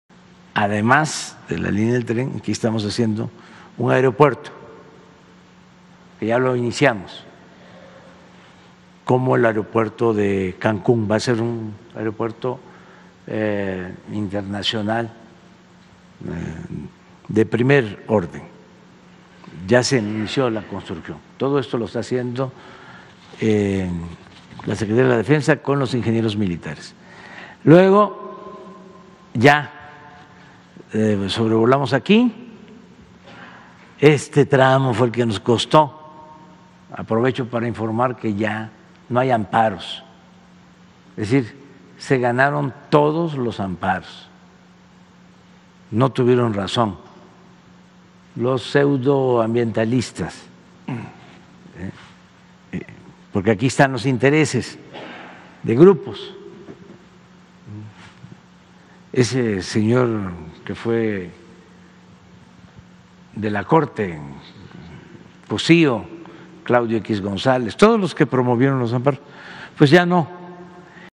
Se ganaron los amparos del Tramo 5 del TrenMaya, no tuvieron razón los pseudoambientalistas: AMLO
"Se ganaron todos los amparos del Tramo 5 del TrenMaya, no tuvieron razón los pseudoambientalistas", celebra López Obrador al señalar al exministro José Ramón Cossío y al empresario Claudio X. González